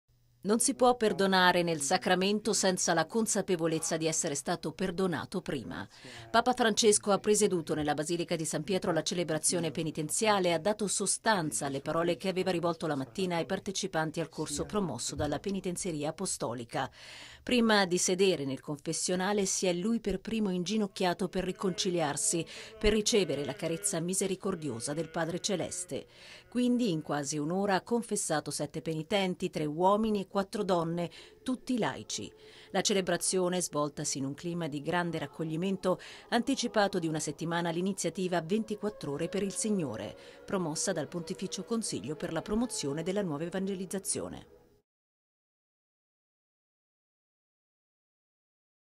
Celebrazione penitenziale 17.03.2017
Il Pontefice si è confessato e ha confessato sette penitenti

Papa Francesco ha presieduto, nella Basilica Vaticana, la celebrazione della penitenza. Un lungo momento di silenzio e raccoglimento ha preceduto le confessioni individuali.